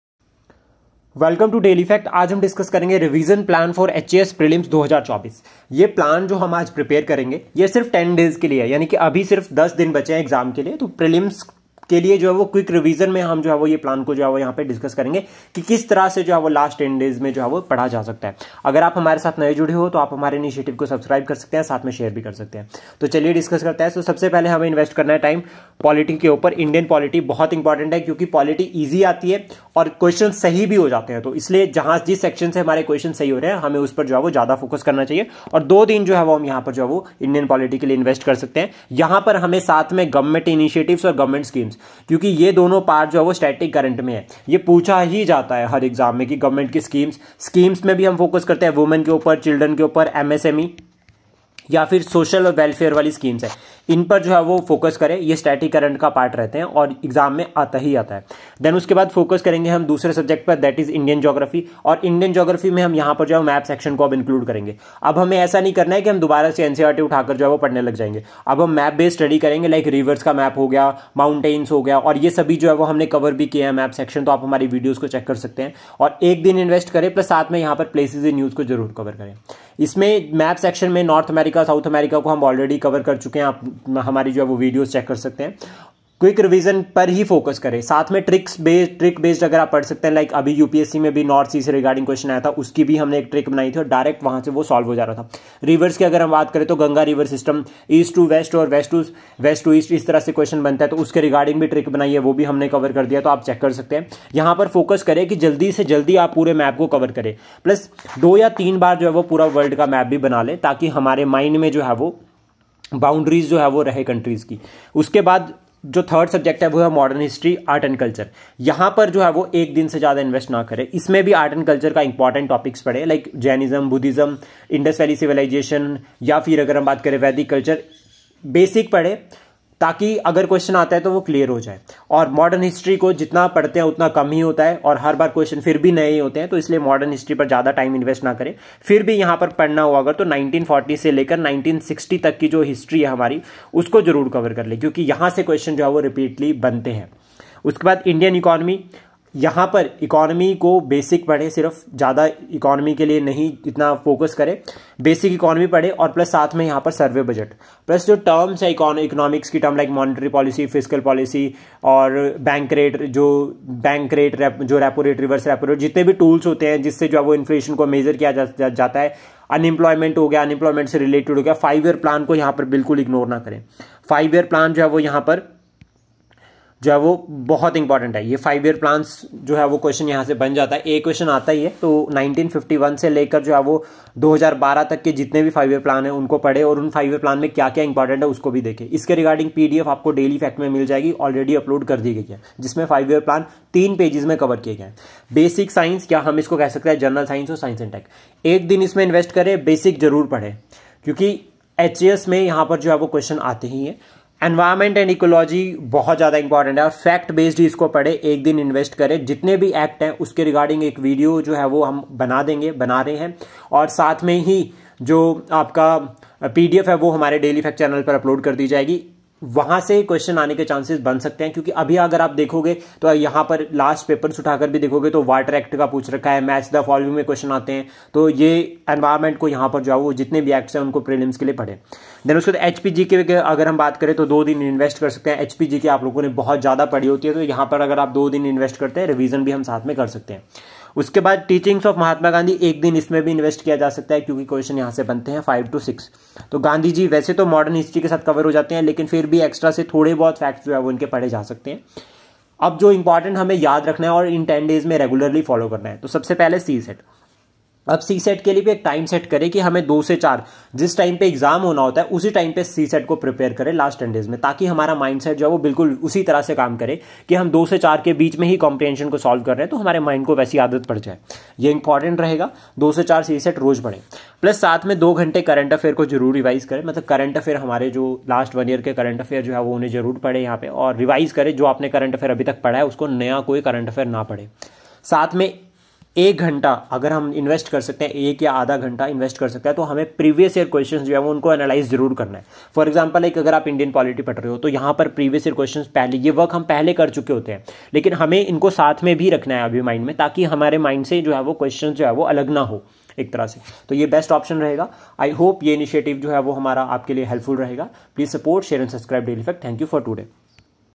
HP HAS Revision Plan for last ten Days II Daily facts II Boost Your Preparation
Complete Revision Plan for Last Ten Days
How to revise History and Current affairs